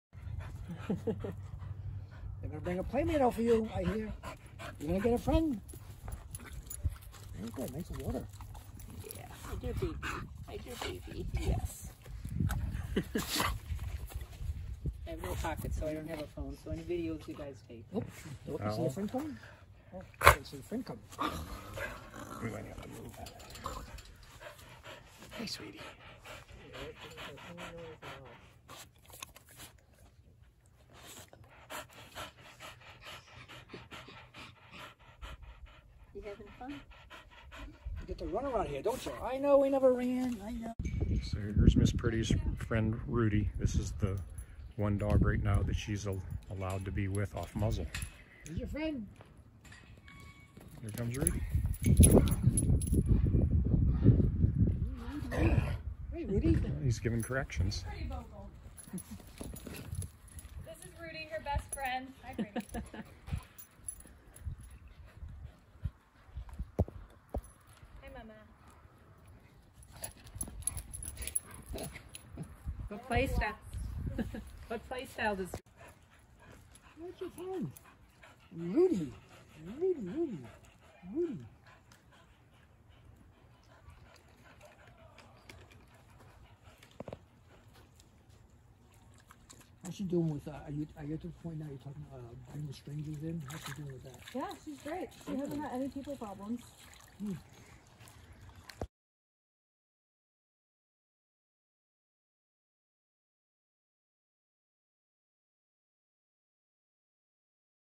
1/20/23 shelter dog Miss Pretty at the training center
Shelter dog Miss Pretty has been at Your Humane Society SPCA since November 12, 2019.  It was suspected she had been used involved in some way with illegal dog fighting based on old scars and a missing piece of ear.  Somewhat wary of some people and seemingly aggressive towards other dogs, no one wanted to adopt her.  Due to her dog aggression and fearing that she would be considered for euthanasia, a GoFundMe fund was started to pay for her to go for behavior modification training and she was transported there November 18, 2022, almost 3 years to the day since she came to the shelter. On January 20, 2023, a few volunteers visited her to see how she was doing and were amazed at her progress.  She has exceeded expectations and will complete her training in a few weeks and be made available for adoption.